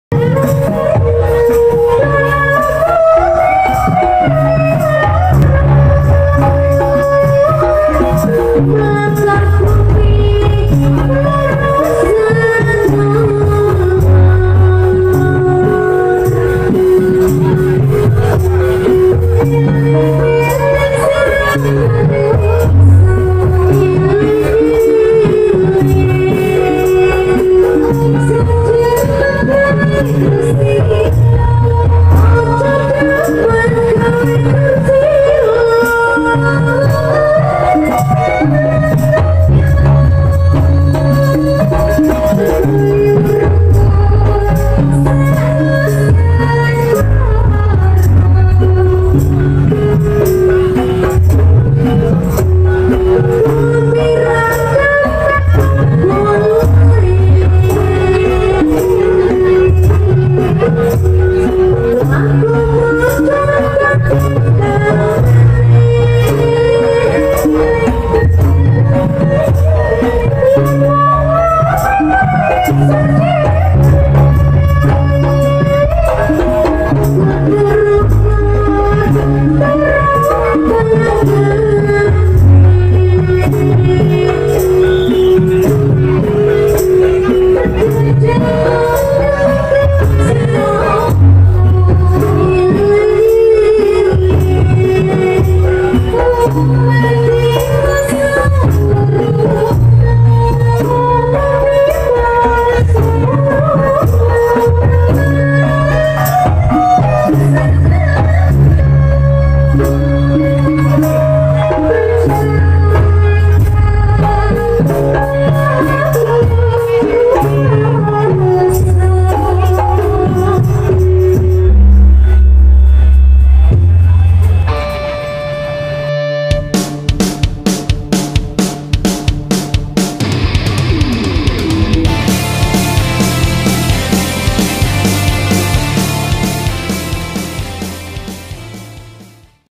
Ndolalak DEWI ARUM TERBARU MANTAPZ !! MEMPESONA ( LIVE lempongsari banyumudal -Sapuran ) Wonosobo
Setelah sekian lama, Kali ini Ndolalak DEWI ARUM dapat Tampil MEMPESONA ( LIVE lempongsari banyumudal -Sapuran ) Wonosobo, dengan menjalankan protokol kesehatan. Semoga dapat menghibur pecinta seni semua. Like dan Coment agar channel ini dapat selalu berkembang dan bermanfaat untuk kita semua. Amin.. amin..